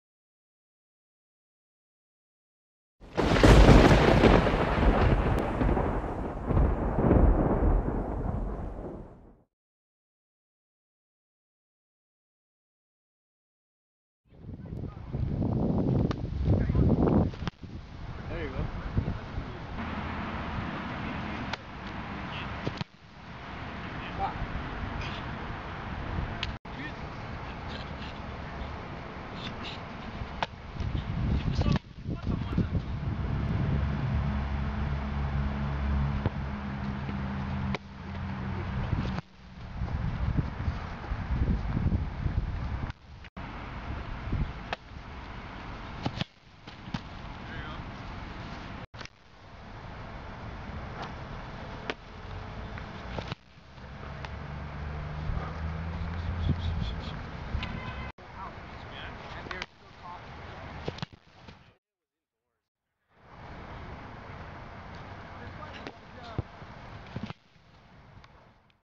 Mike Renaud Punter April 25 2011 Workout - Presented by Centre 68
Mike Renaud (Winnipeg Blue Bomber)  Punter April 25 2011 Workout  with Mike Harrigton Long Snapping - Presented by Centre 68